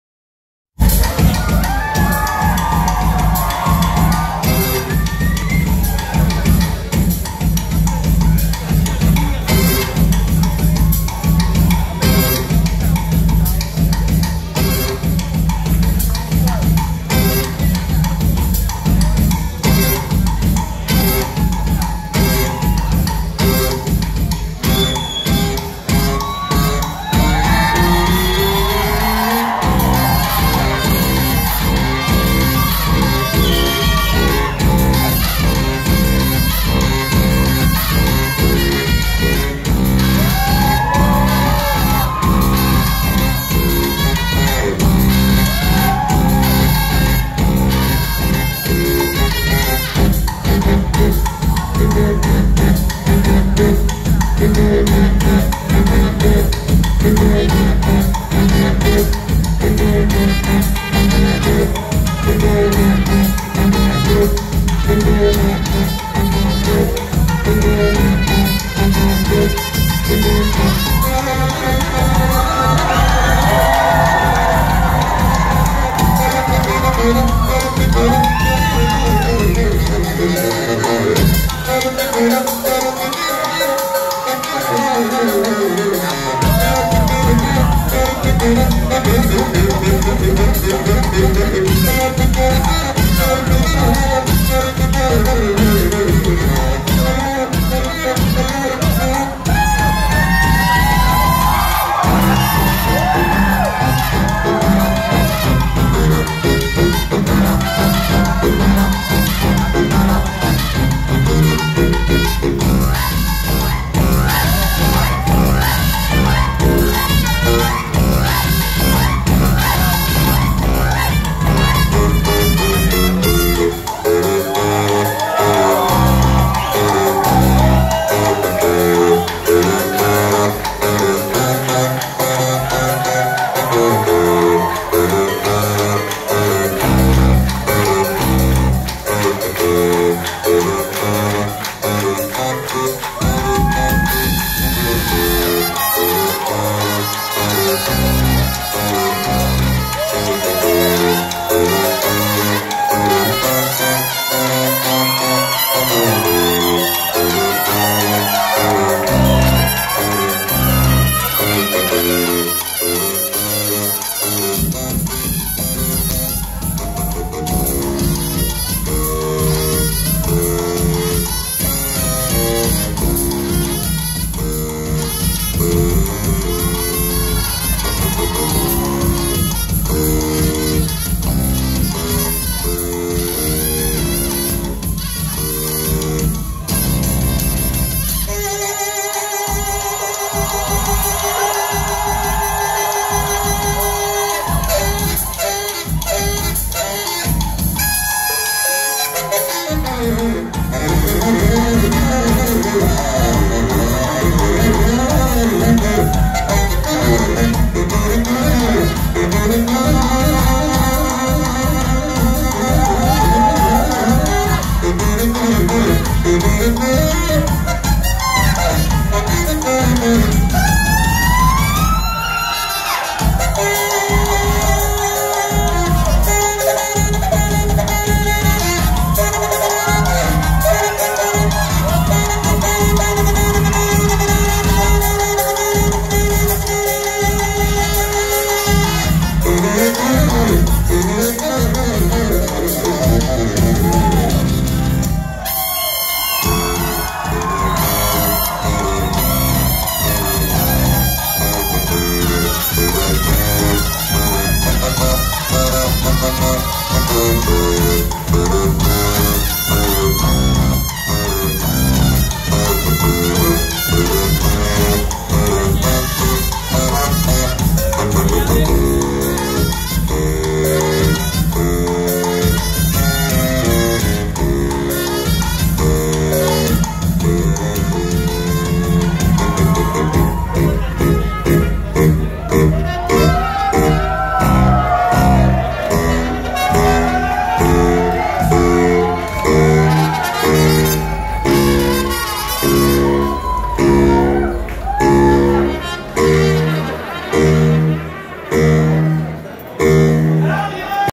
Too Many Zooz - Prague 2018
Too Many Zooz in Roxy club - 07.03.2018 Prague, Czech Republic.